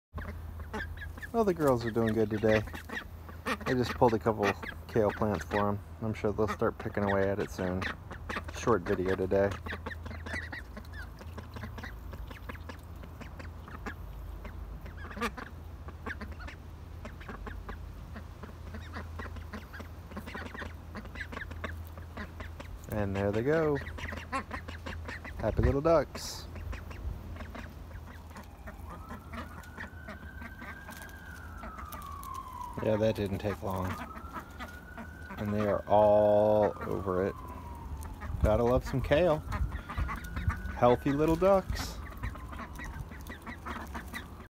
Day 41 - Kale From The Community Garden
Well it's day 41 and I needed to pull up a few kale plants from the garden to make room for other veggies that are maturing and the goats were not out, so I brought them home for the girls to enjoy.  Healthy Little Ducks!